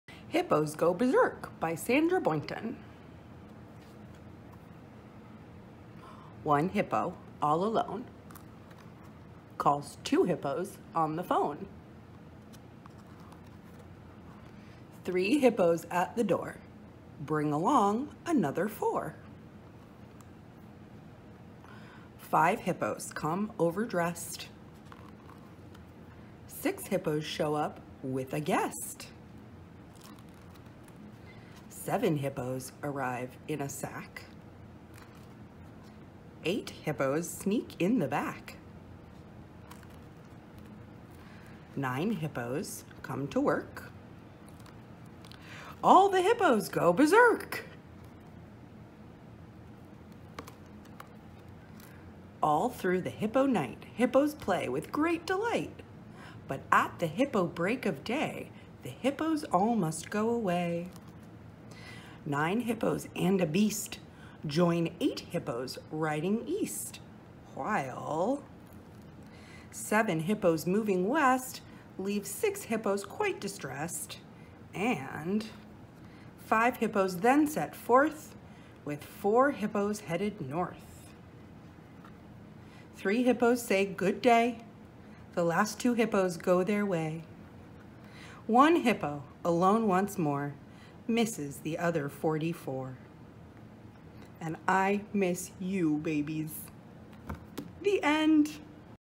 Sarah reads Hippos go Berserk!
Enjoy listening along to Sarah, as she reads Hippos go Beserk!